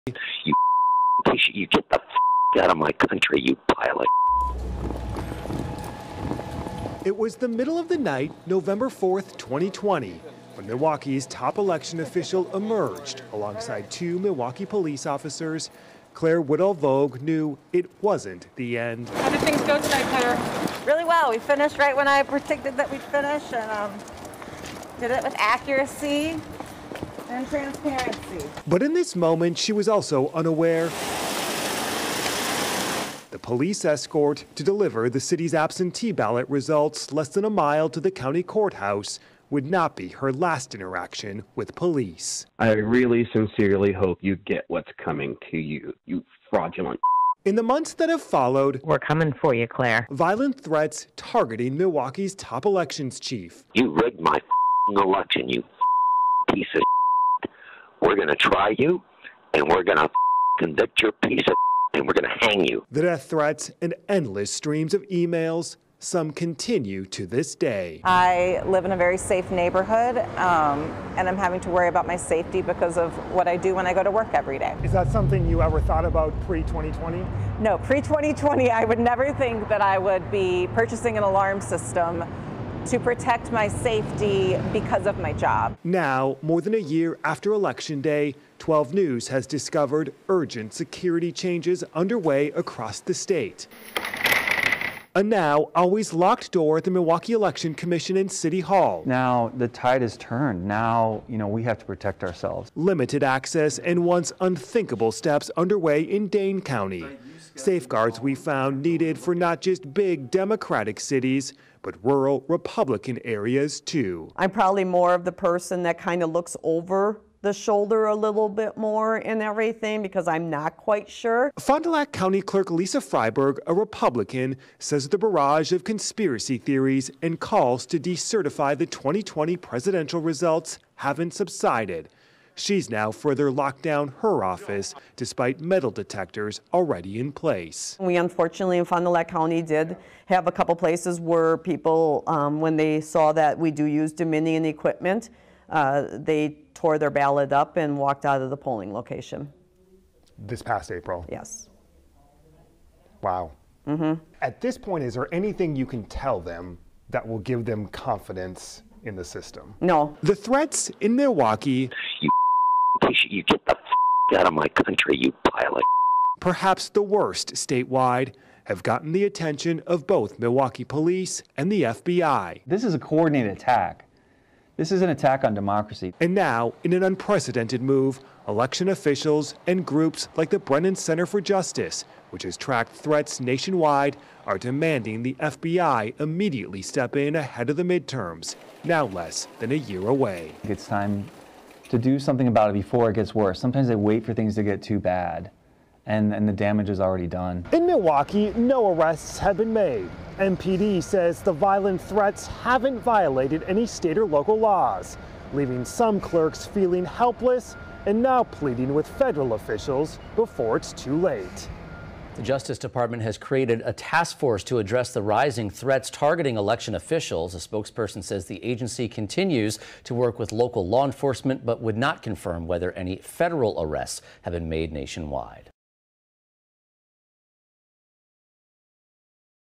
Wisconsin Election Officials Face Death Threats | RepresentUs
Wisconsin election officials—from both parties—are facing death threats for doing their jobs. Help support them and protect Wisconsin elections from partisan takeover

We’ve help pass over 156 anti-corruption laws and resolution, from anti-gerrymandering and ranked choice voting to sweeping ethics and transparency laws. In this RepresentUs video titled Wisconsin Election Officials Face Death threats, we're exposing the ongoing harassment election workers in Wisconsin are still receiving after the 2020 election. Extremists are attempting to bully these civic workers out of their jobs so they can takeover future presidential elections and local elections in the state.